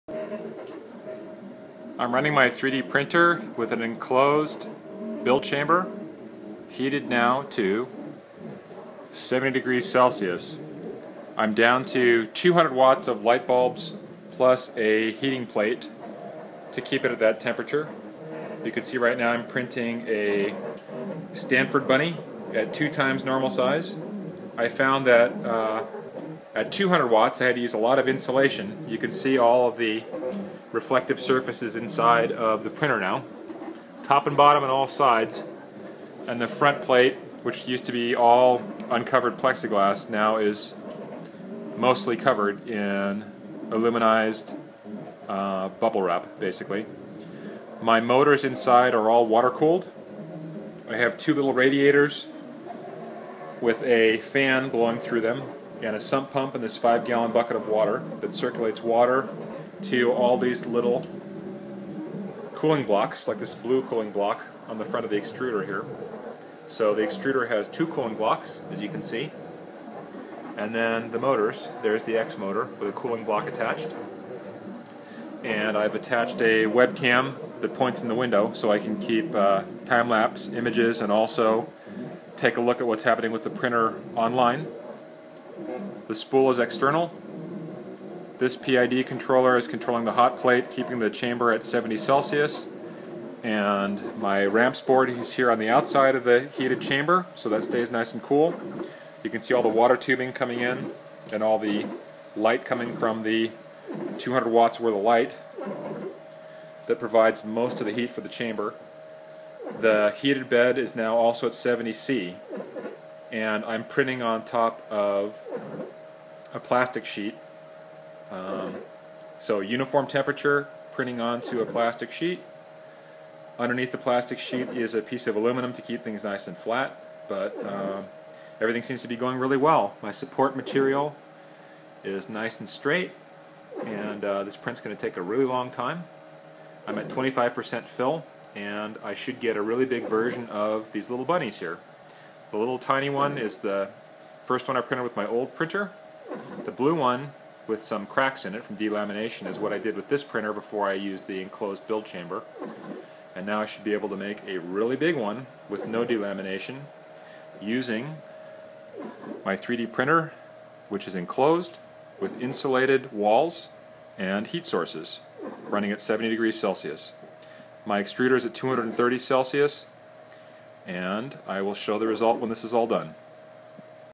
Heated, insulated build chamber 3D printer
70 C insulated build chamber. 70 C heated platform. 200W light source and heated plate maintain temperature.